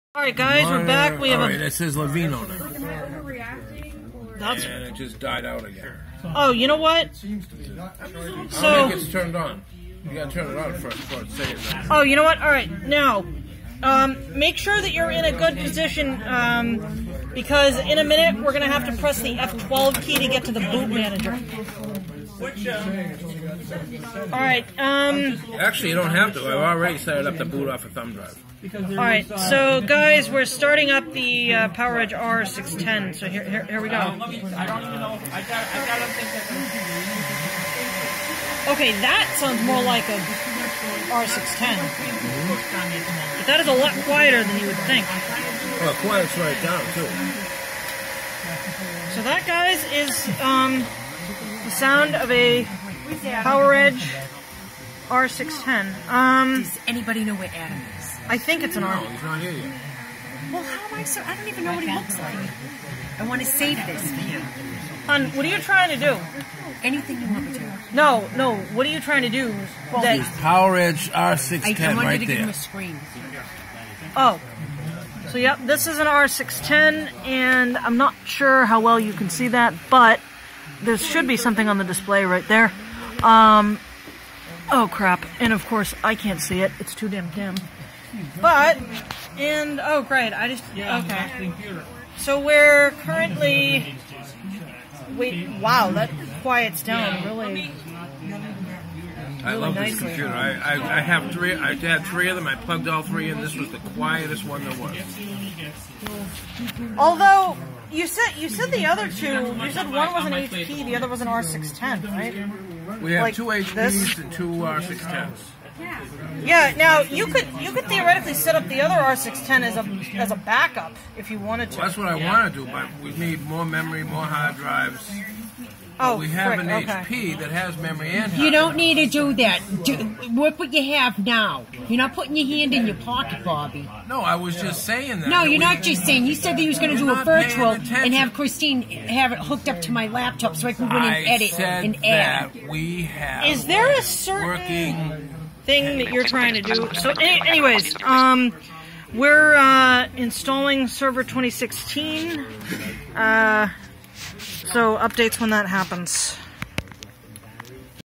Startup of the PowerEdge R610
I decided to start up the unit on video for you guys. It's amazing how quiet these things are!